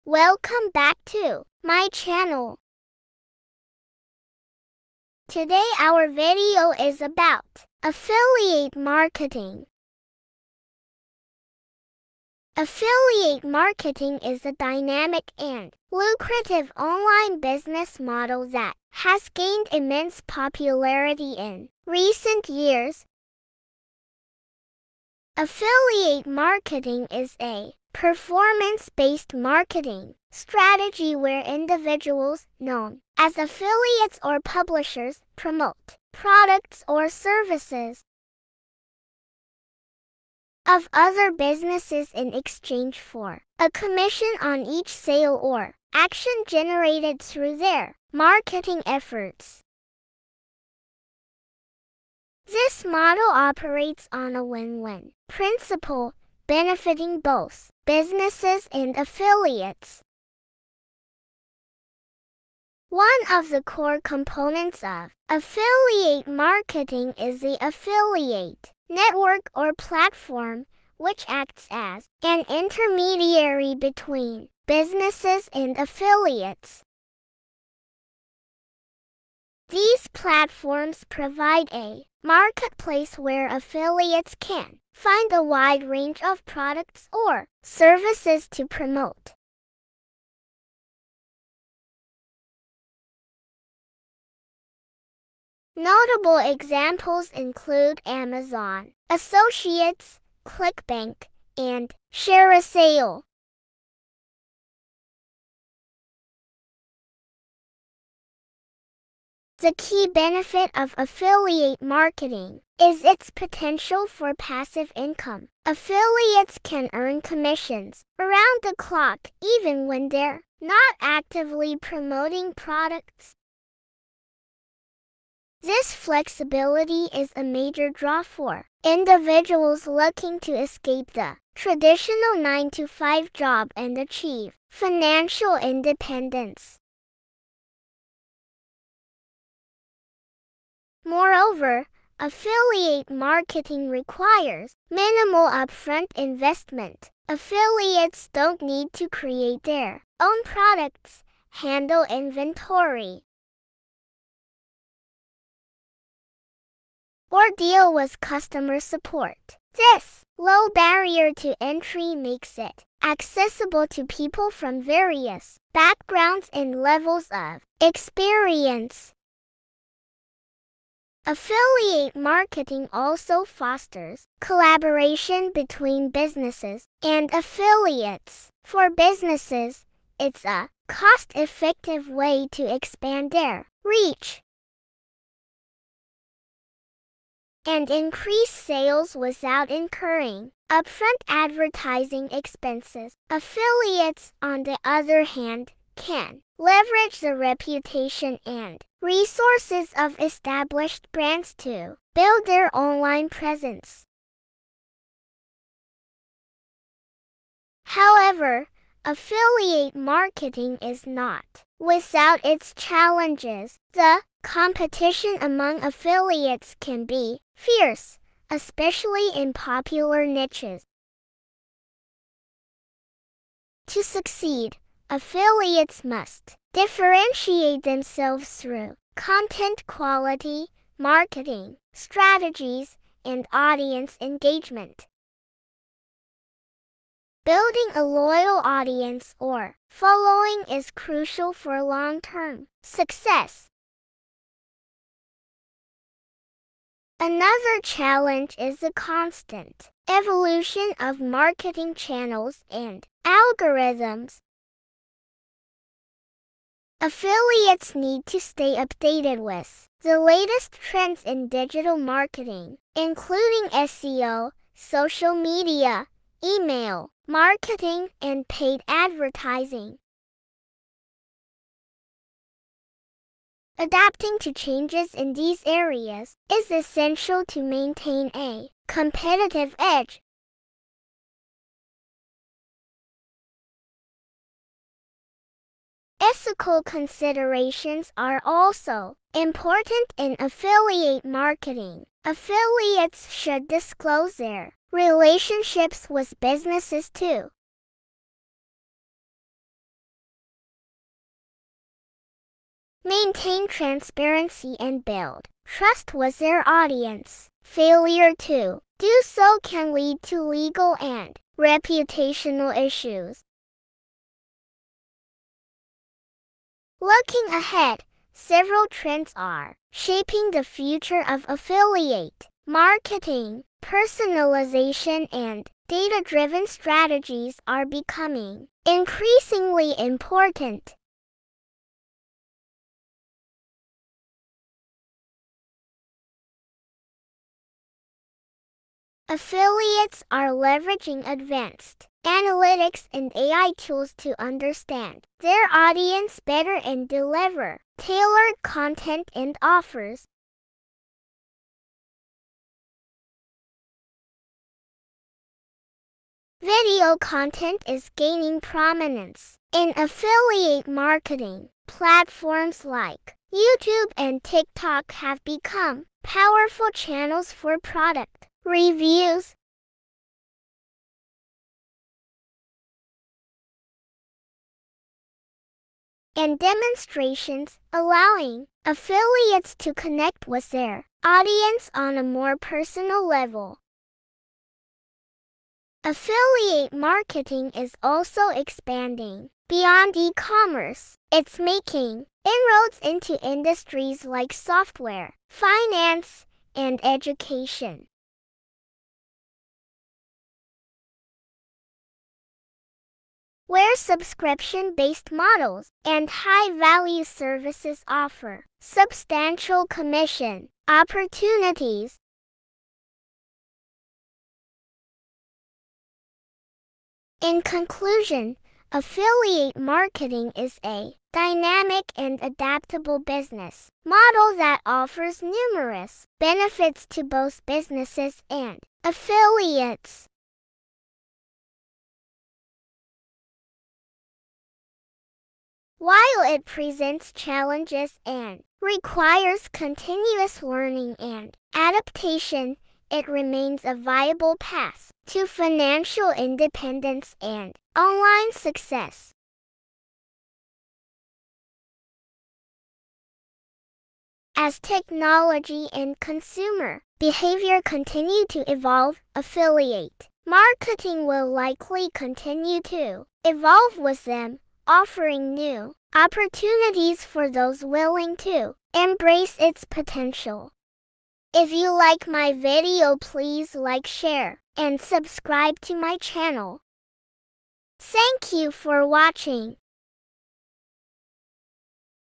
The Power of Affiliate Marketing: Turning Clicks into Cash
What is Affiliate marketing / The Ultimate Guide to Affiliate Marketing Success

Affiliate Marketing From Little to no Money | Affiliate Alchemy: Turning Clicks into Cash

Affiliate Marketing
Diversifying Income Streams
To build a sustainable affiliate marketing business, it's important to diversify your income streams. This can include
promoting a variety of products and services, as well as exploring different affiliate networks and platforms.
Consider promoting both physical and digital products to reach a wider audience.
Explore affiliate networks beyond Amazon, such as CJ Affiliate, ShareASale, and Rakuten Marketing.

Creating Long-Term Partnerships📌

Building long-term partnerships with brands can provide a stable source ofy in a compelling way.
Engage with the brand and their audience on social media to build relationships and trust.
to
Negotiate for higher commission rates based on your performance and the value you provide to the brand.